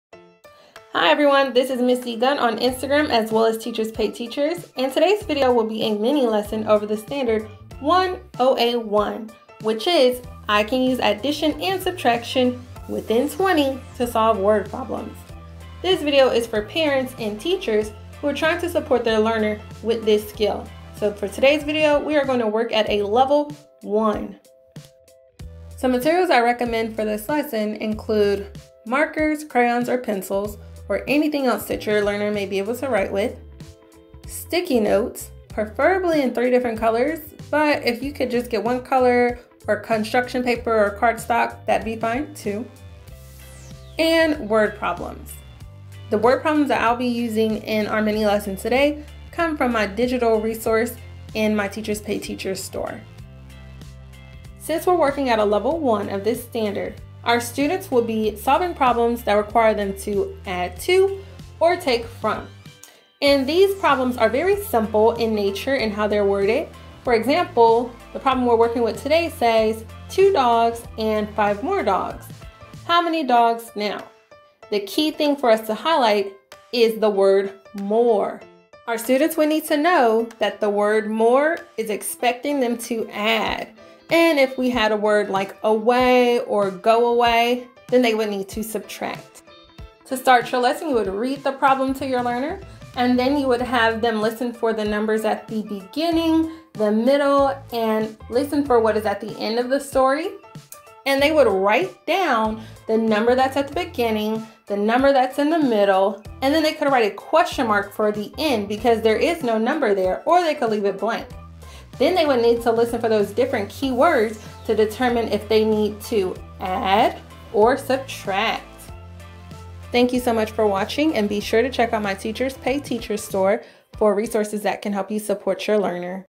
Word Problems | First Grade Add to and Take From (1.OA.1) | Miss D. Gunn
♡ 1.OA.1 Word Problems 1st Grade ♡ mathlessons Parents & Teachers watch this standards-based lesson and learn how to support children with this solving word problems & problem solving in math.

This lesson can be done at home or at school. Great for families homeschooling, new teachers, and teachers simply needing lesson ideas!

  ♡♡♡♡♡♡ OTHER FREE GOODIES ♡♡♡♡♡♡

 ♡♡♡ TEACHER FRIENDS TO WATCH ♡♡♡

⚫Facebook: Miss D. Gunn ♡page♡

 ♡♡♡♡♡♡ BUSINESS INFO. ♡♡♡♡♡♡